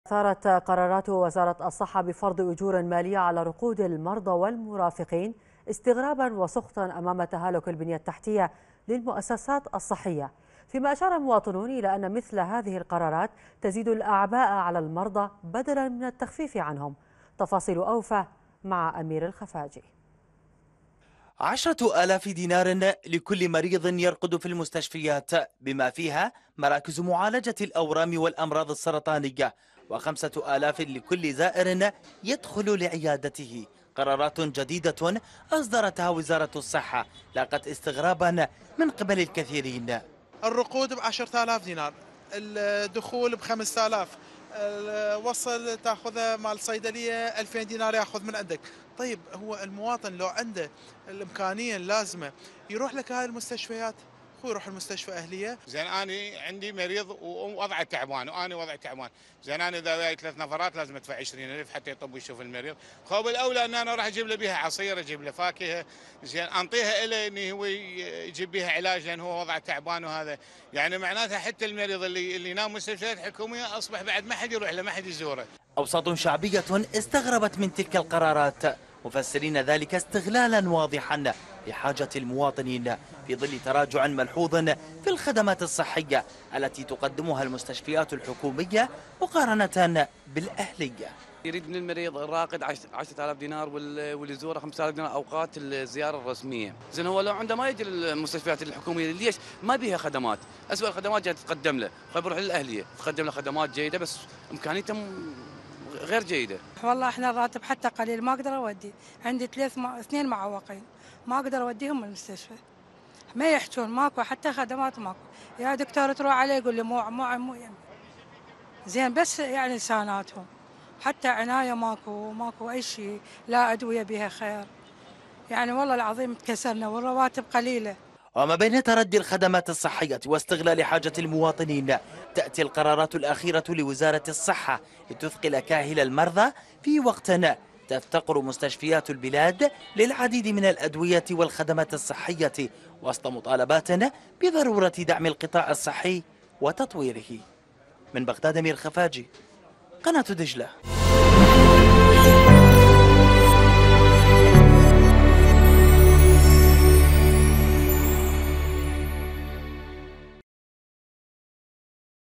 انتقادات لقرارات الصحة بفرض أجور مالية على رقود المرضى في المشافي
11258📺 افقي